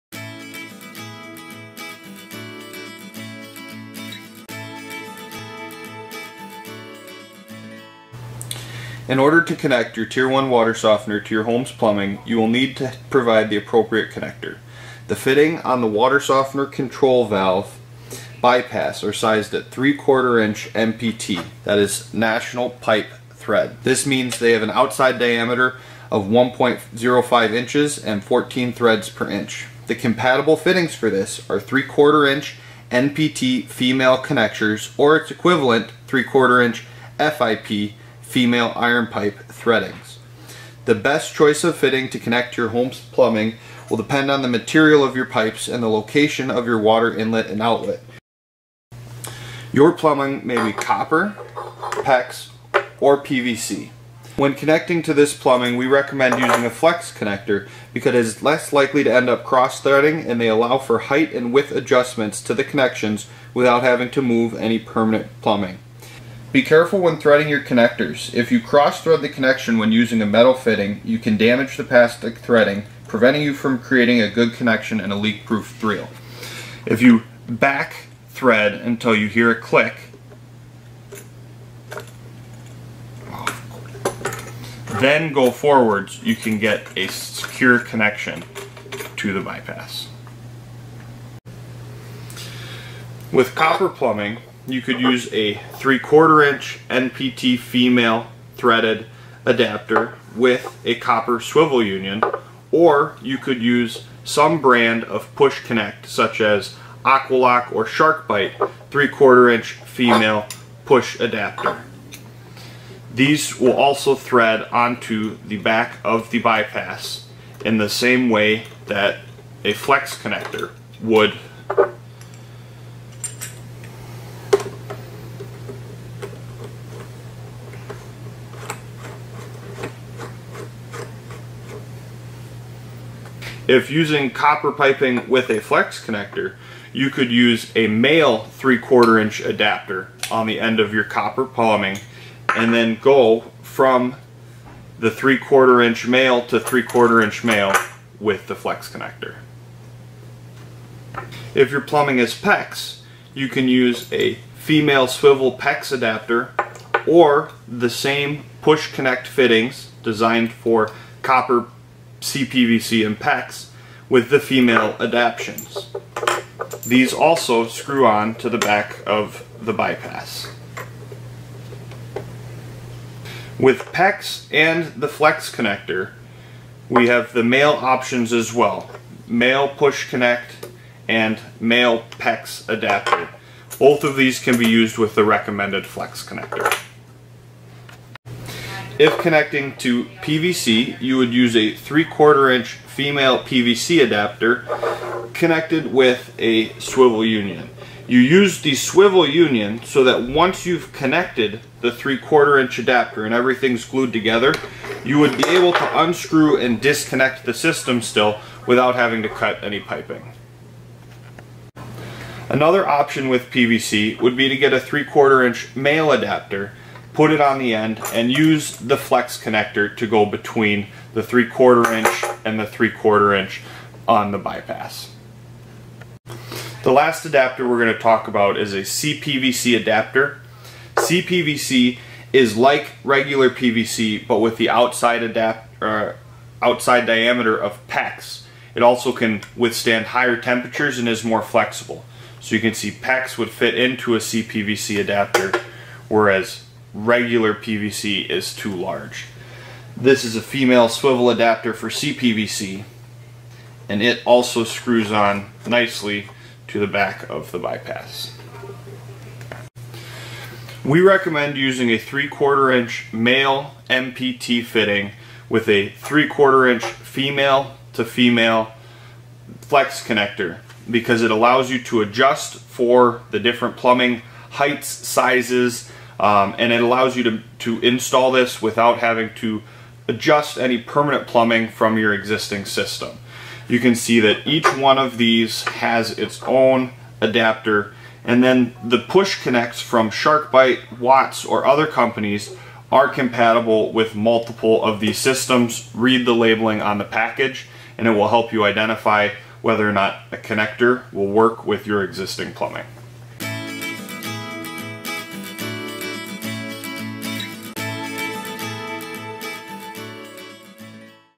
How to Choose Plumbing Connections for Your Tier1 Water Softener
This video explains what type of plumbing connections can be used to connect to the Tier1 165 Water softener.

This video may be helpful prior to or during the installation process.

Key moments:
0:00:18 Bypass fitting size
0:00:31 Compatible fitting size
0:00:50 Types of plumbing
0:01:19 Threading flex connector
0:01:31 Fittings for copper plumbing
0:02:13 Flex to 3/4" copper MNPT adapter
0:02:31 Fittings for Pex plumbing
0:02:53 Flex to 3/4" Pex MNPT or push to connect
0:03:08 Fittings for PVC plumbing
0:03:31 Flex to PVC MNPT
0:03:47 Fittings for CPVC plumbing
0:04:20 Plumbing Fitting Recommendations